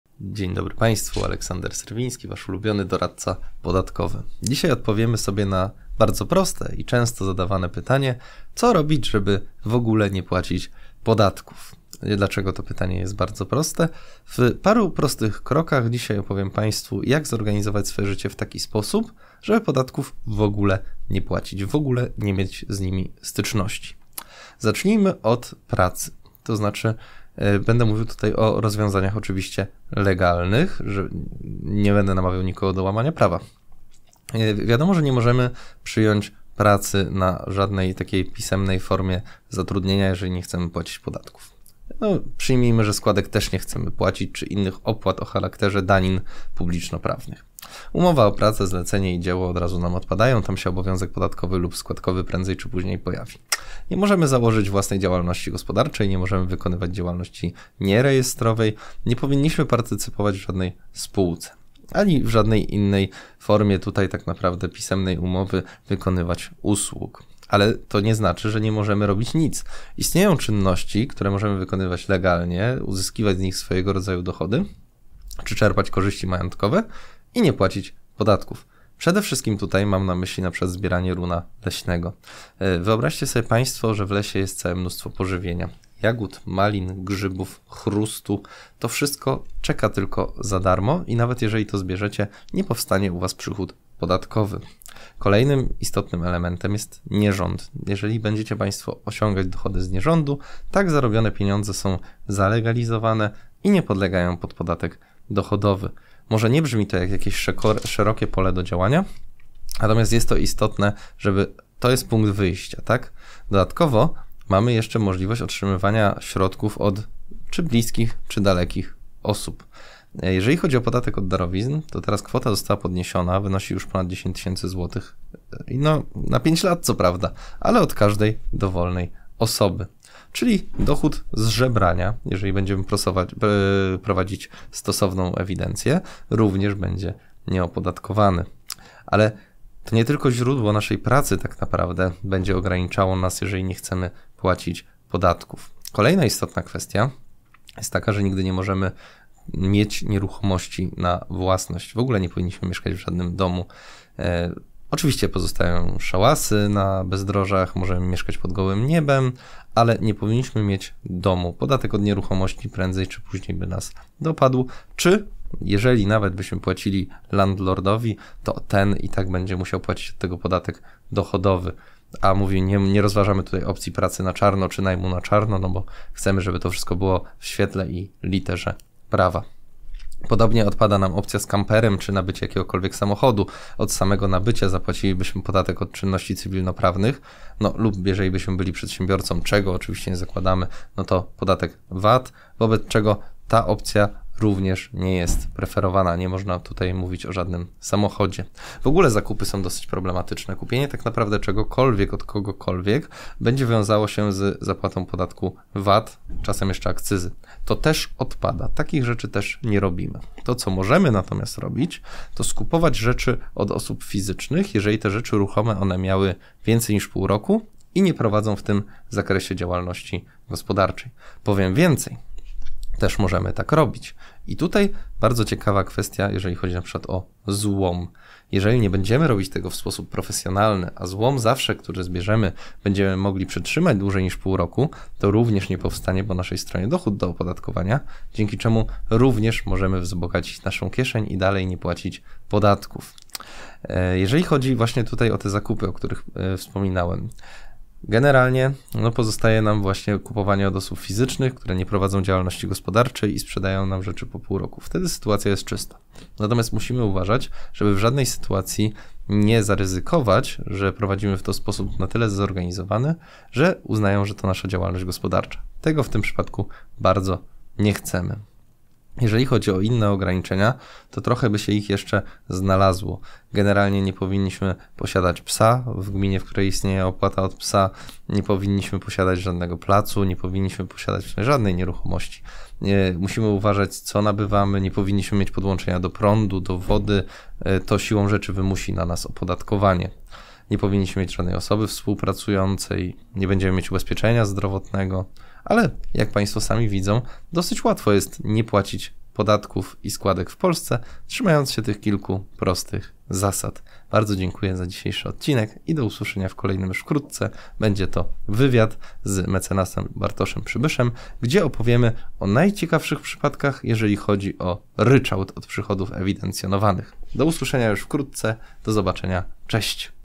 Jak łatwo nie płacić podatków?
Czy naprawdę płacenie podatków jest smutną koniecznością w XXI wieku? Czy istnieją rzeczy, które są wolne od opodatkowania? Czy współczesny człowiek może żyjąc zgodnie z prawem nie płacić żadnych podatków?

Na to pytanie spróbuję odpowiedzieć wskazując kilka praktycznych porad. Jest to dużo prostsze, niż większość osób myśli.